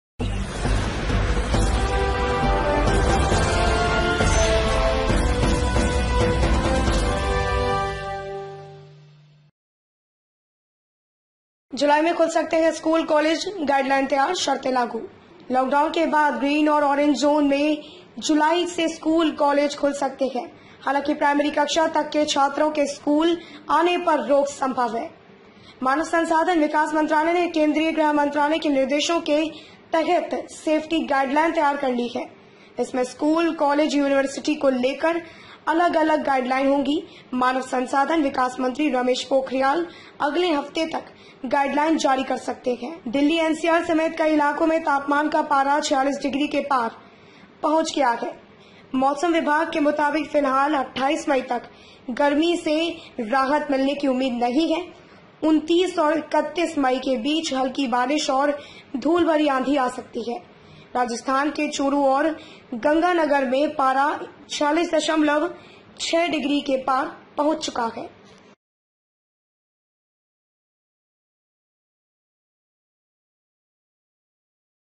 जुलाई में खुल सकते हैं स्कूल-कॉलेज, गाइडलाइन तैयार, शर्तें लागू
बड़ी खबर: जुलाई में खुल सकते हैं स्कूल-कॉलेज, गाइडलाइन तैयार, शर्तें लागू

नई दिल्‍ली। लॉकडाउन के बाद ग्रीन और ऑरिज जोन में जुलाई से स्कूल-कॉलेज खुल सकते हैं। हालांकि, प्राइमरी कक्षा तक के छात्रों के स्कूल आने पर रोक संभव है।
मानव संसाधन विकास मंत्रालय ने केंद्रीय गृहमंत्रालय के निर्देशों के तहत सेफ्टी गाइडलाइन तैयार कर ली हैं। इसमें स्कूल, कॉलेज, यूनिवर्सिटी को लेकर अलग- अलग गाइडलाइन होगी। मानव संसाधन विकास मंत्री रमेश पोखरियाल अगले हफ्ते तक गाइडलाइन जारी कर सकते हैं।

Breaking news
दिल्ली-एनसीआर समेत कई इलाकों में तापमान का पारा 46 डिग्री के पार पहुंच गया। मौसम विभाग के मुताबिक फिलहाल 28 मई तक गर्मी से राहत मिलने की उम्मीद नहीं है।  29 और 31 मई के बीच हल्की बारिश और धूलभरी आंधी आ सकती है राजस्थान के चूरू और गंगानगर में पारा 46.6 डिग्री के पार पहुंच चुका है।